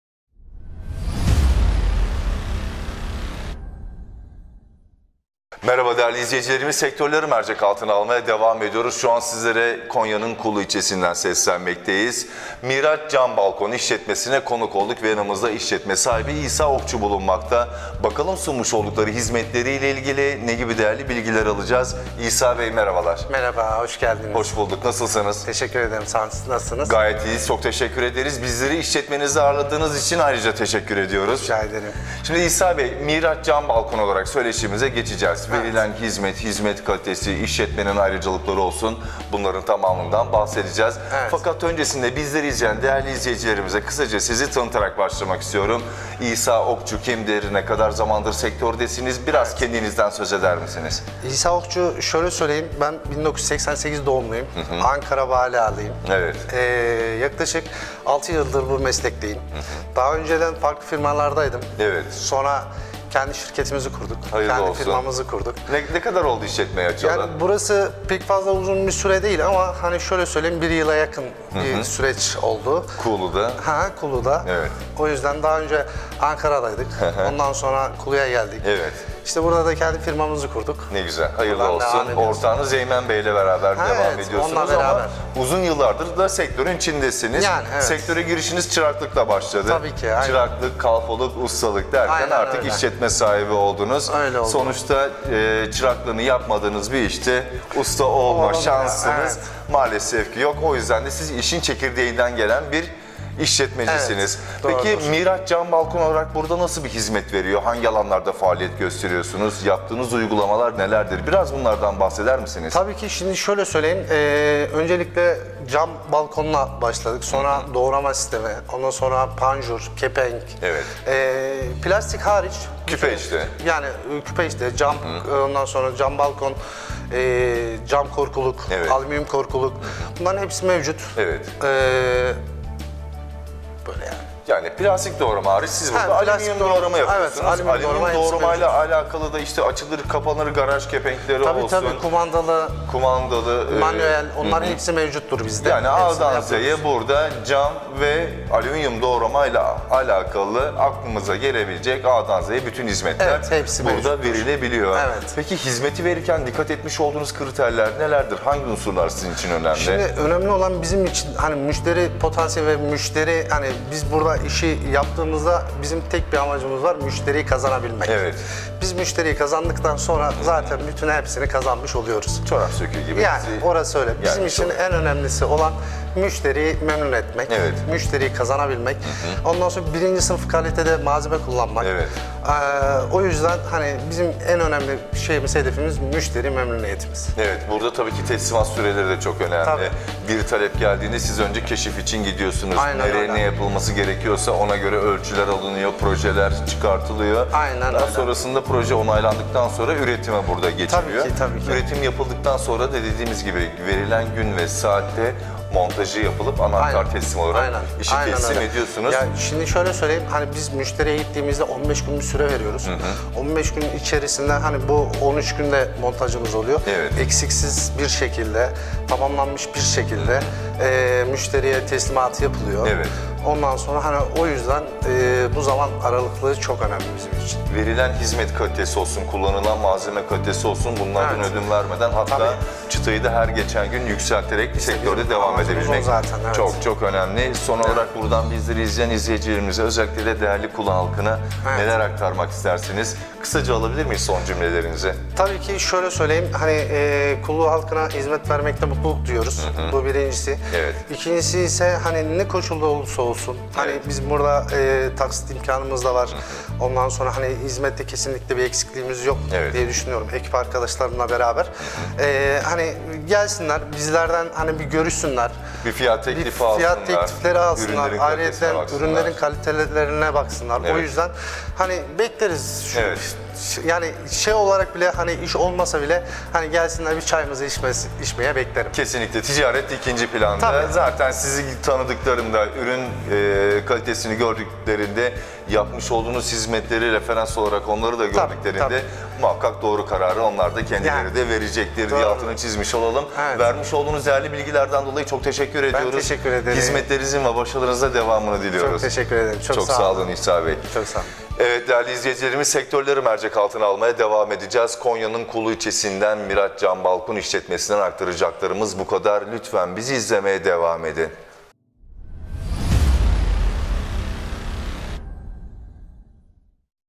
MİRAÇ CAM BALKON SİSTEMLERİ - Kulu / KONYA
MİRAÇ CAM BALKON SİSTEMLERİ işletme sahibi İsa OKÇU, başarı ve faaliyetlerini Konya Haber TV muhabirimiz Cemal YERGİN' e anlattı.